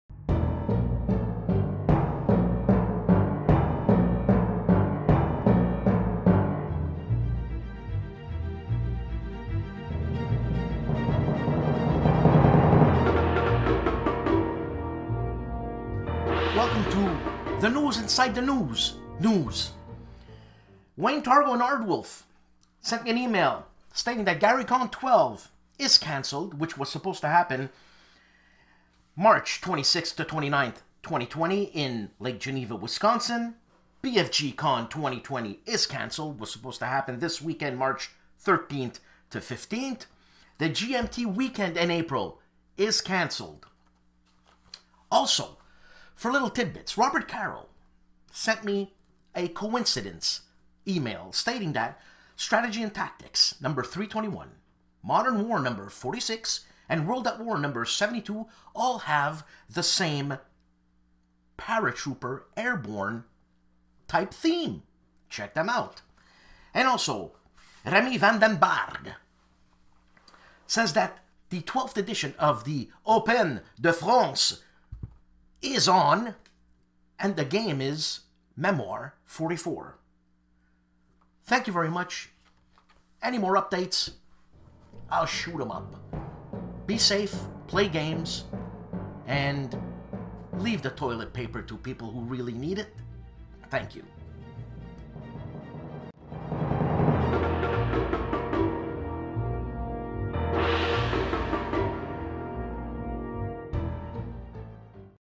Inside the News
News about people who review, unbox, showcase, do playthroughs, podcasts and more concerning war board games. Some music too.
TIME STAMP

ROBERT CARROLL
Evolution of Historical Small Unit Tactical Board Games.